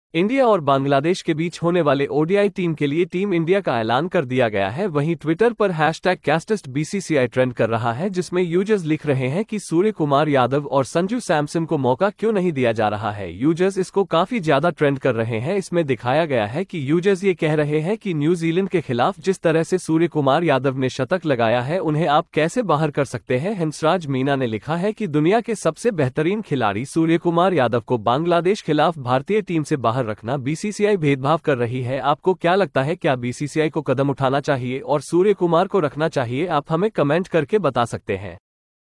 Surya and Sanju not selecting in odi against Bangladesh | Surya and Sanju not selecting controversy
Team India has been announced for the ODI team to be held between India and Bangladesh, while Casteist_BCCI is trending on Twitter, in which users are writing why suryakumar yadav and sanju samson are not being given a chance.
Users are trending it a lot, it has been shown that users are saying that how can you exclude the way Suryakumar Yadav has scored a century against New Zealand
 Hansraj Meena has written that keeping the world's best player Suryakumar Yadav out of the Indian team against Bangladesh is discrimination by BCCI. What do you think, should BCCI step in and keep Suryakumar? You can tell us by commenting.